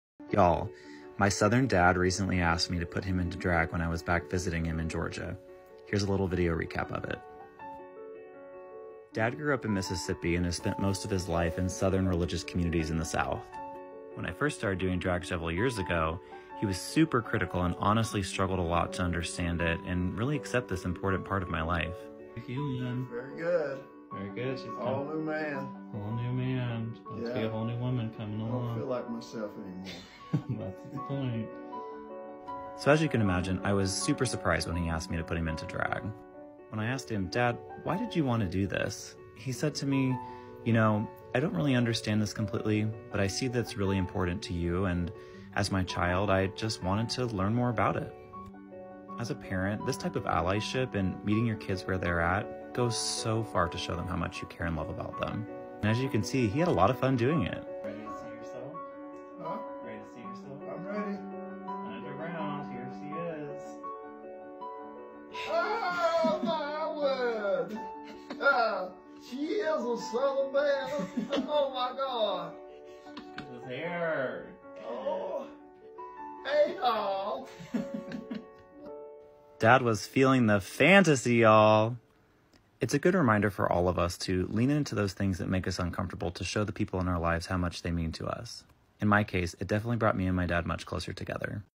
Gay Life: Professional Drag Queen Miss Mary Lou Pearl transforms his Str8 Southern Father into Drag
from TikTok - from @  missmaryloupearl
Professional Drag Queen Miss Mary Lou Pearl transforms his Str8 Conservative Southern Father into Drag.  Why?  Because his father Loves him very much!   ❤️💜